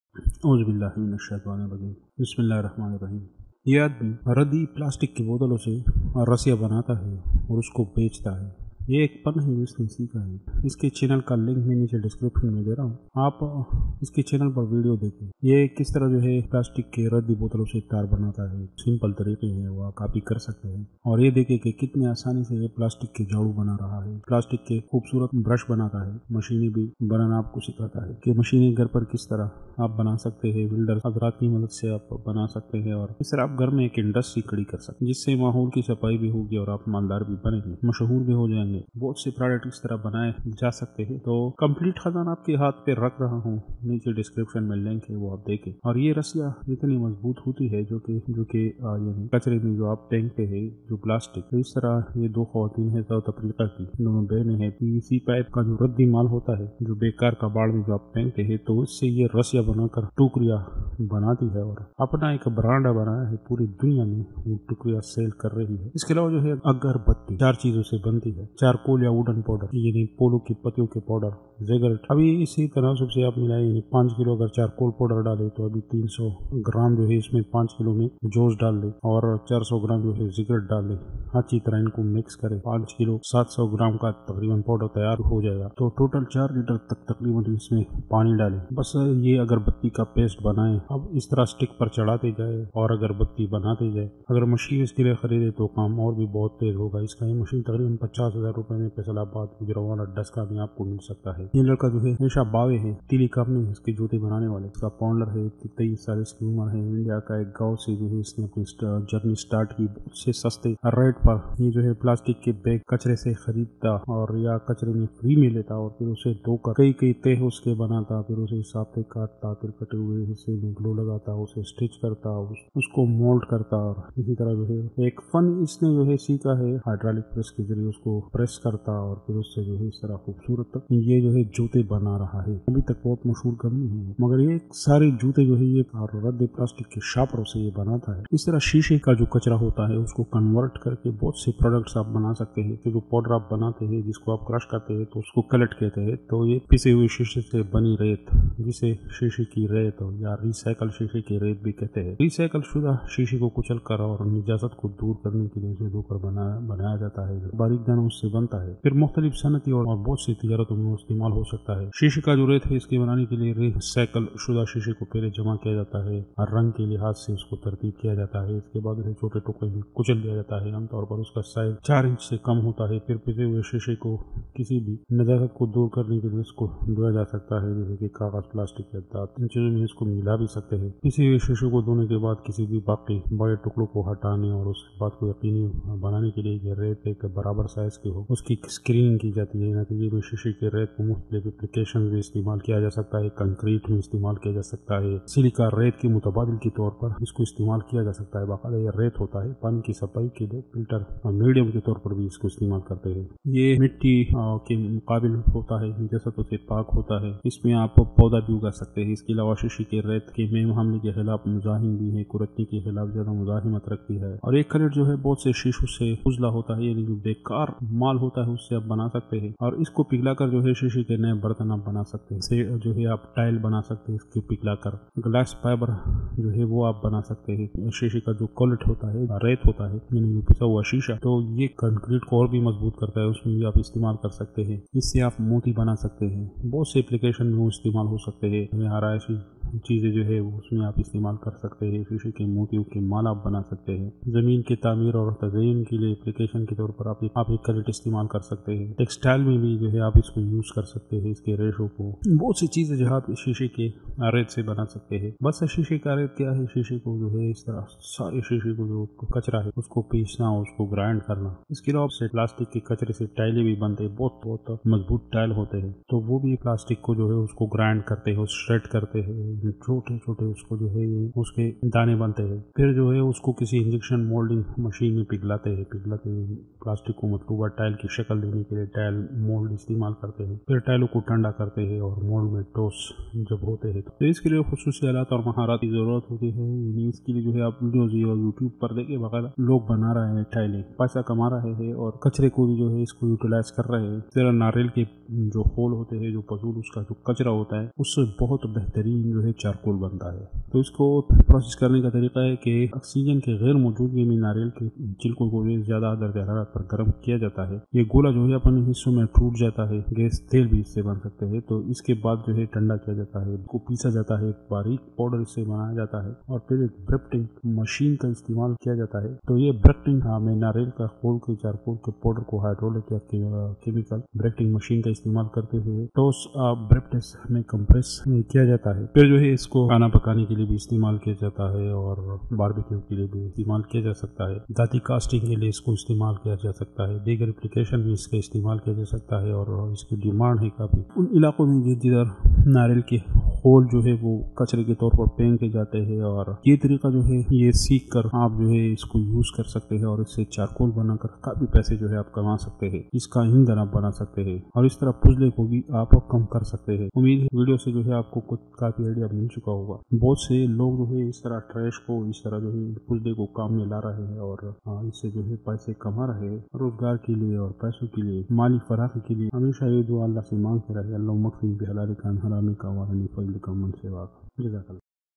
7 smelly businesses that can make you a millionaire in Urdu and Hindi
7 smelly businesses that can make you a millionaire in Urdu and Hindi
Best Business Ideas Urdu Hindi